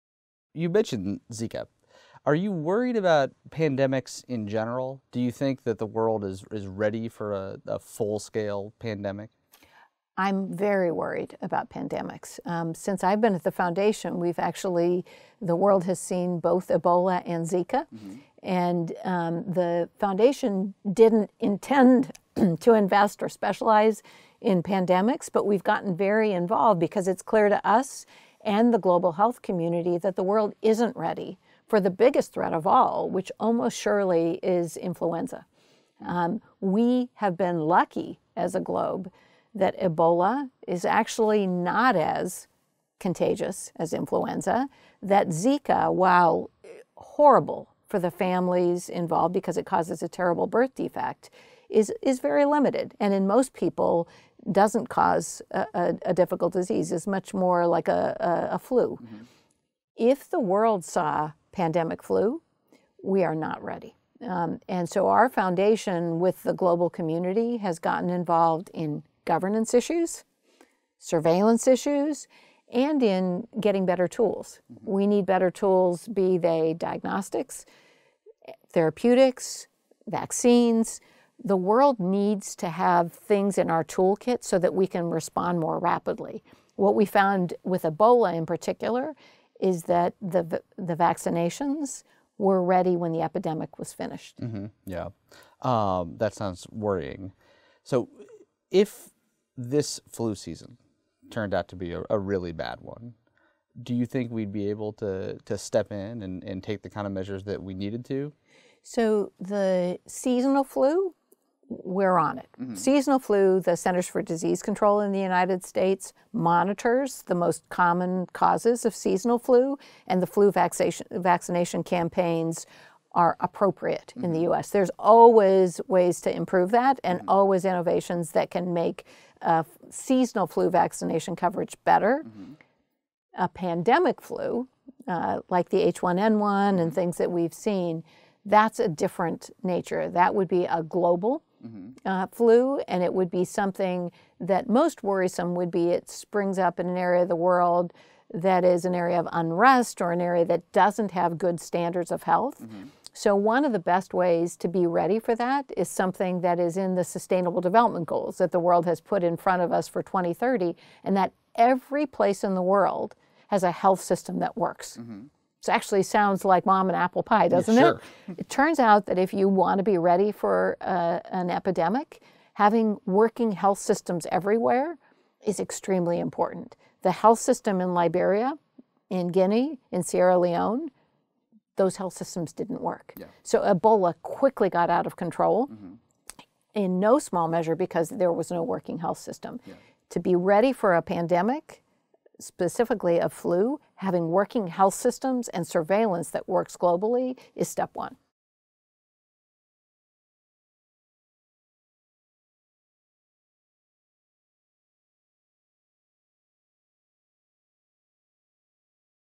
Sue Desmond-Hellmann: Is the world prepared for a full-scale swine flu pandemic? | VIEWPOINT
Is the world prepared for a global swine flu pandemic? "No" says Sue Desmond-Hellmann, CEO of the Bill & Melinda Gates Foundation. Michael Strain, AEI's Director of Economic Policy Studies and Resident Scholar, finds out how we can stop a global flu before it happens.

Thumbnail photo credit:
Reuters